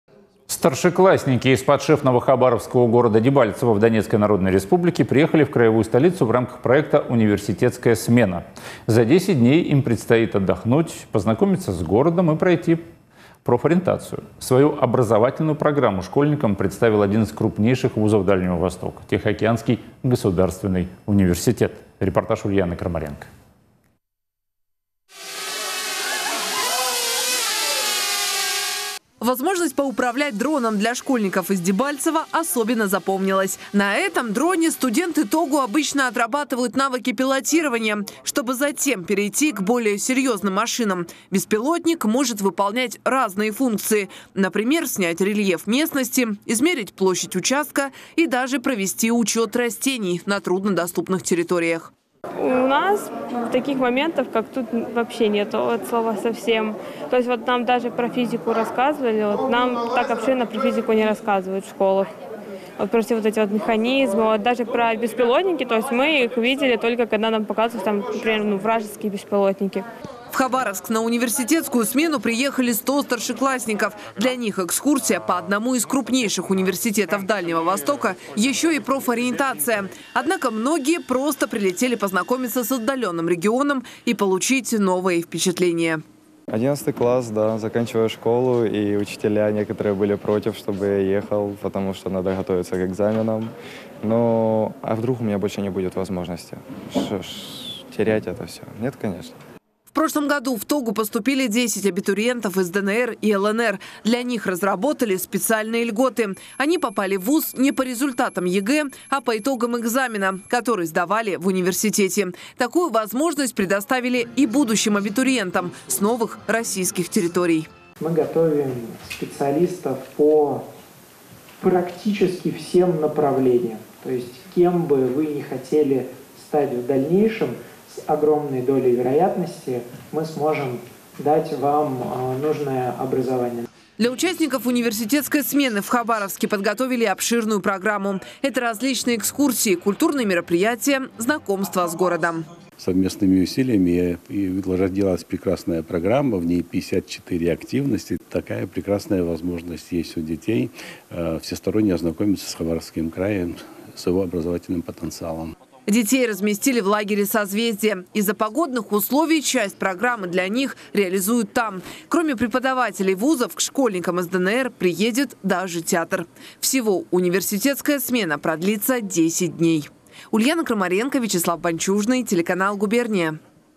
Поездка в Хабаровск: гости из Дебальцево в ТОГУ. Новости. 20/11/2023. GuberniaTV
Старшеклассники из подшефного Хабаровску города Дебальцево в Донецкой народной республике приехали в краевую столицу в рамках проекта "Университетская смена". За 10 дней им предстоит отдохнуть, познакомиться с городом и пройти профориентацию. Свою образовательную программу школьникам представил один из крупнейших вузов ДФО -  Тихоокеанский государственный университет.

ok.ru/guberniatv